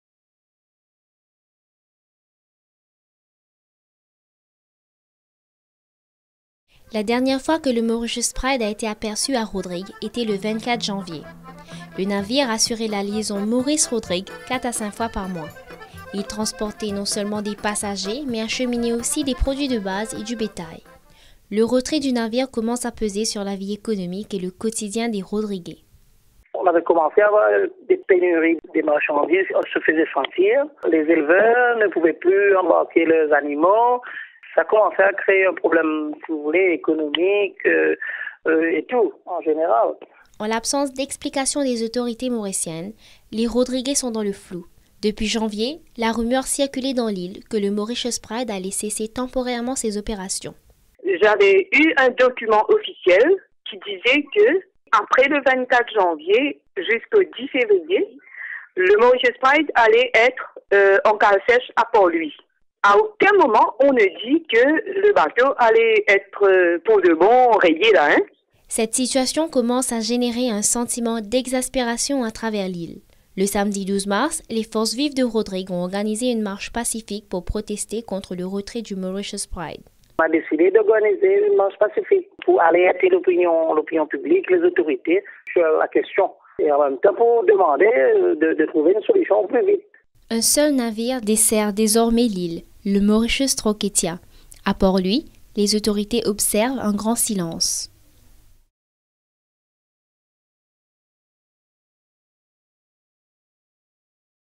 Mauritius Pride à l'arrêt : Rodrigues craint l'isolement
A Rodrigues, c'est ressenti comme un coup dur. Depuis que le Mauritius Pride a cessé de desservir l'île, la vie n'est plus la même pour beaucoup de ses habitants. Certains produits de base commencent à se faire rares, et le bétail s'exporte moins, affirment-ils. Des Rodriguais joints au téléphone s'expriment.